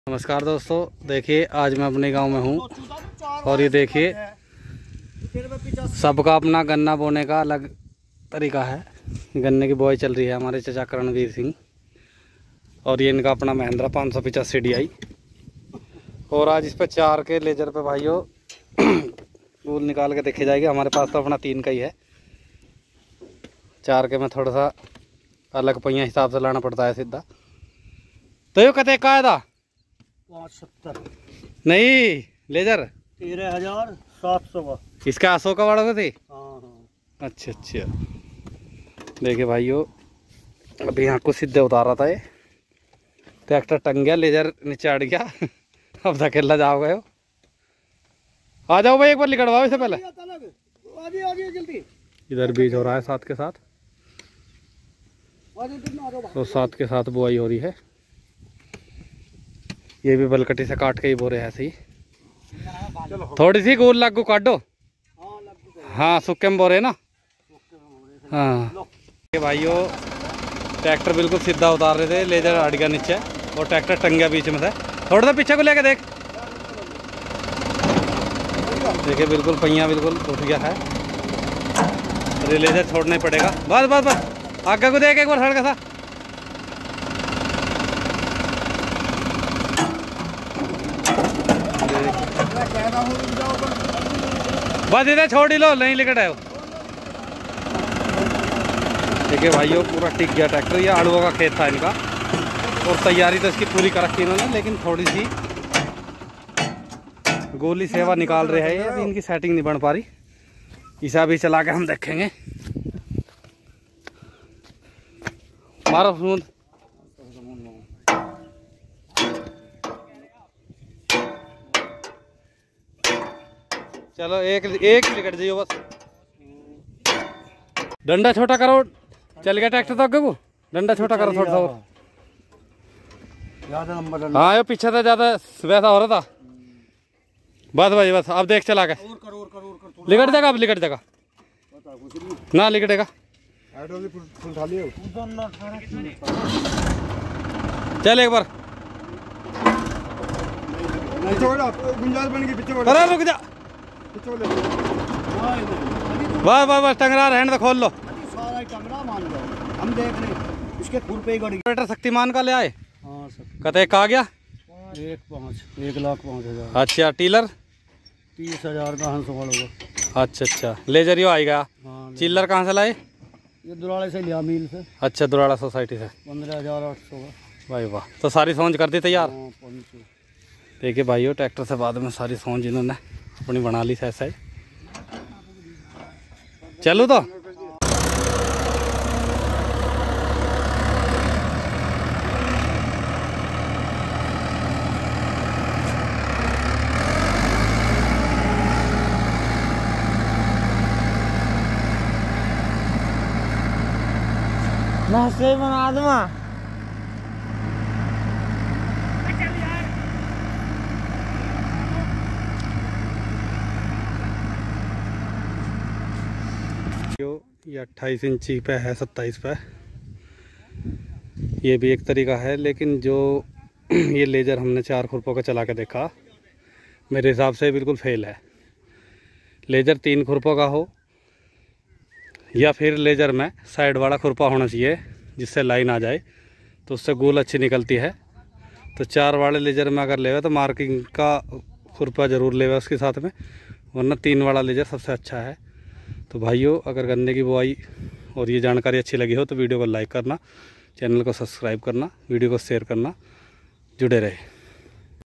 सबका गन्ने की बुवाई का अपना-अपना तरीका 4 खुरपा वाला लेजर है 28 inch गन्ना बुवाई में फेल
इस वीडियो में गन्ने की बुवाई 28 इंची पर सबका अपना अपना तरीका हुआ ही का 4  वाला लेजर फेल